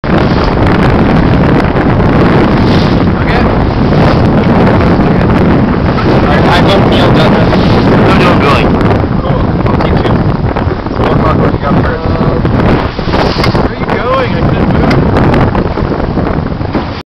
Thunderbird 2012 @ RVYC March 24, 2012
Second on-water testing for Thunderbird 2012.